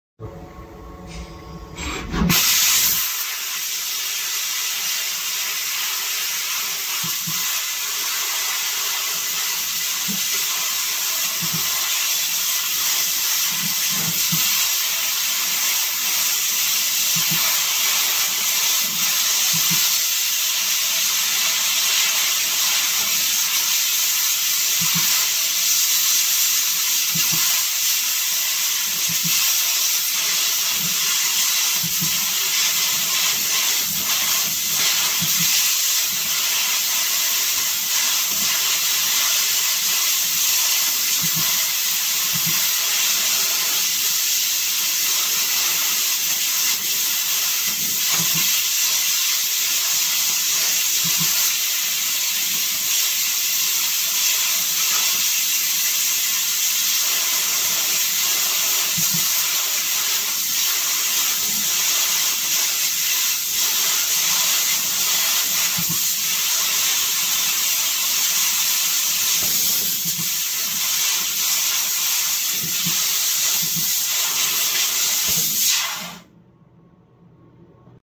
How to cut 2.5mm thick silver plate? Silver and Gold Fiber Laser Cutting Machine
SHENZHEN BEC LASER TECH CO., LTD